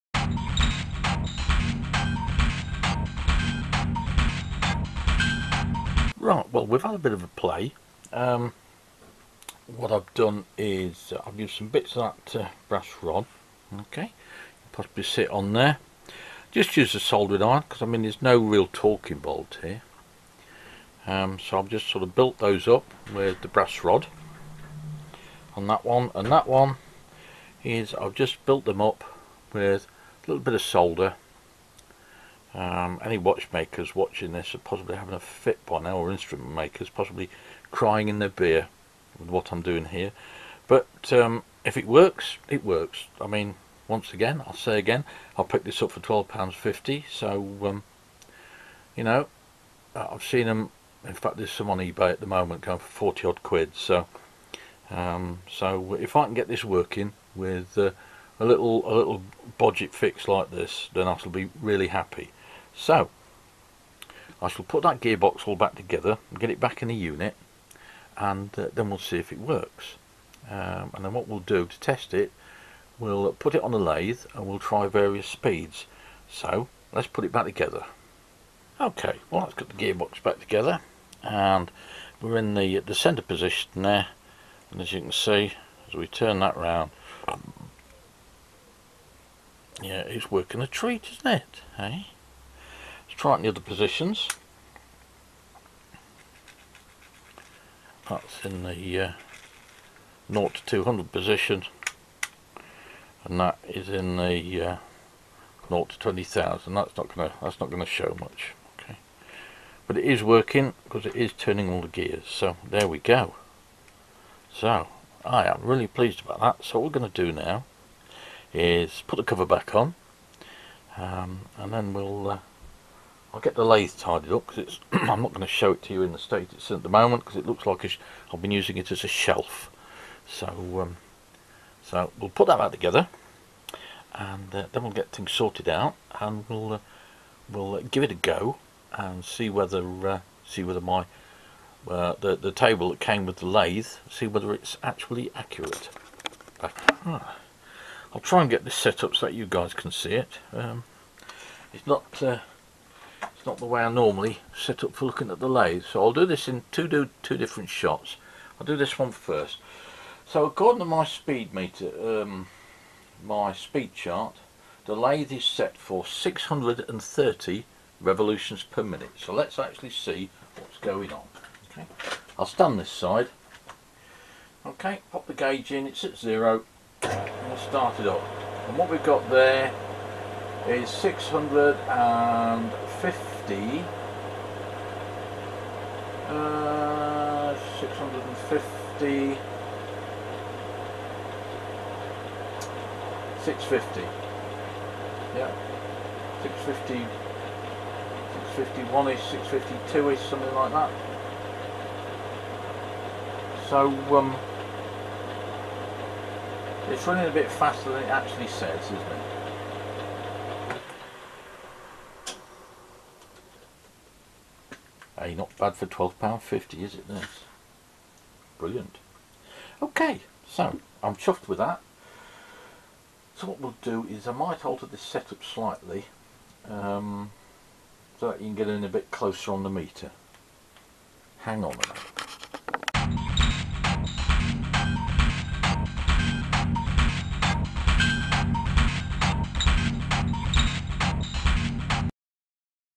Venture Tacho Part 3
3rd of 4 parts describing the teardown and repair of a Smiths Venture ATH7 Mechanical Tachometer